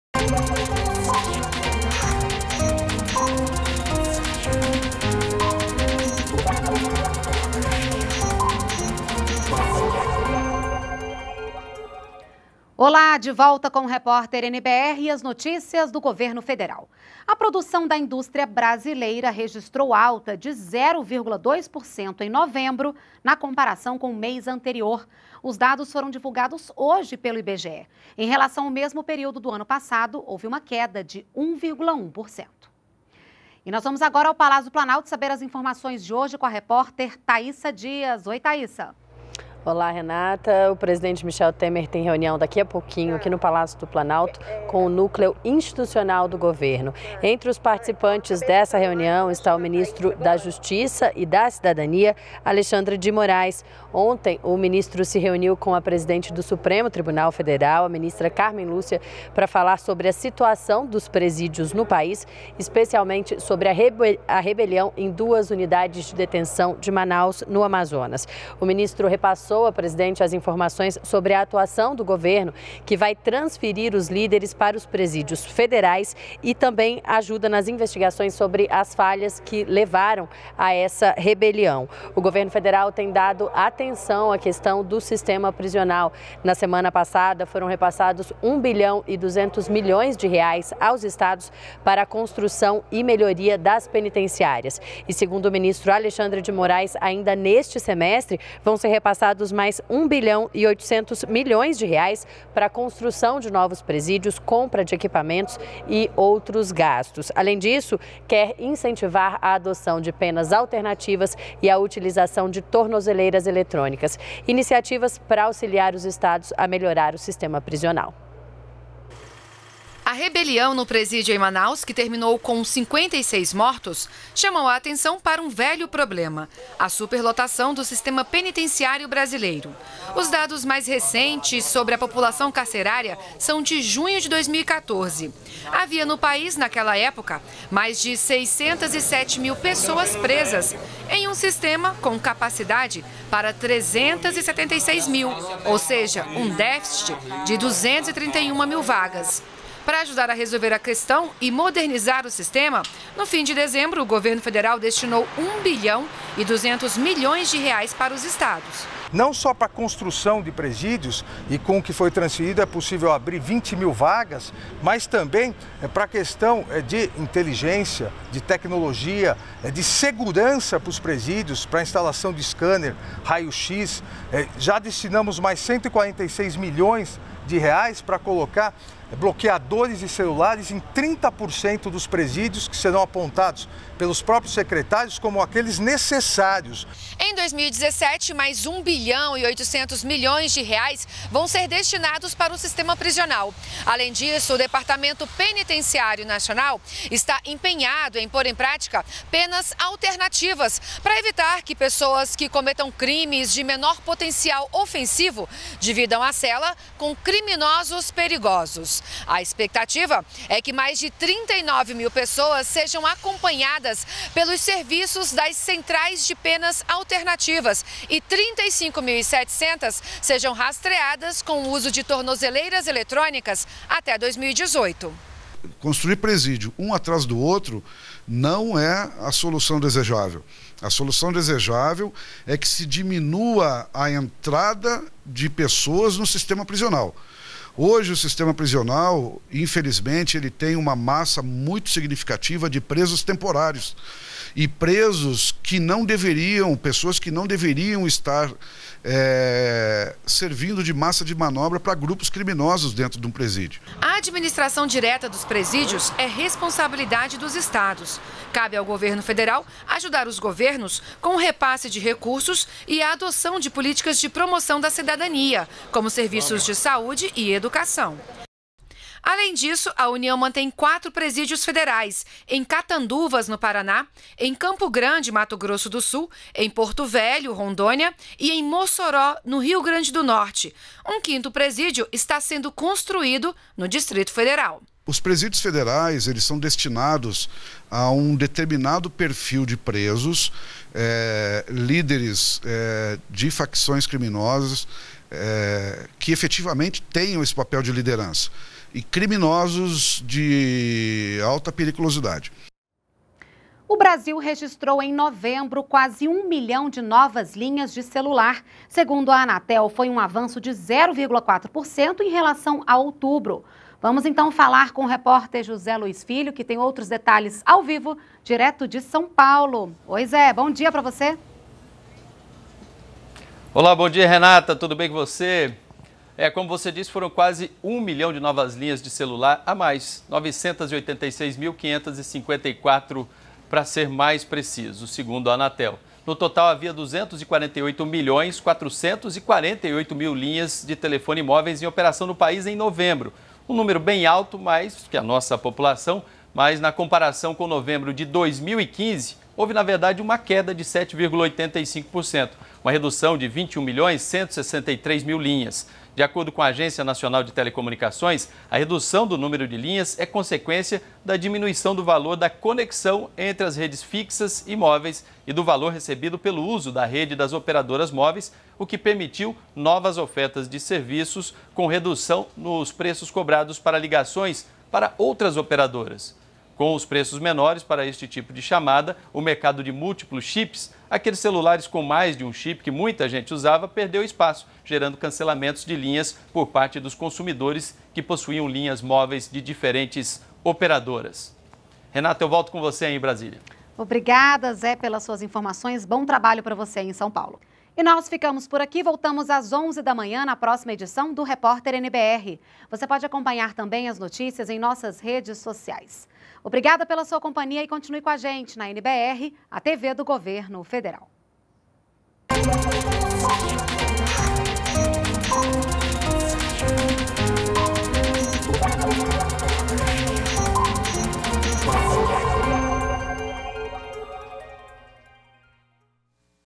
Indústria brasileira registra alta de 0,2% em novembro
REPÓRTER NBR 10H - 05.01.17: Produção da indústria brasileira registra alta de 0,2% em novembro em relação ao mês de outubro 2016. E ainda: Brasil registrou em novembro quase 1 milhão de novas linhas de celulares. Presidente Temer participa de reunião com o núcleo institucional do governo.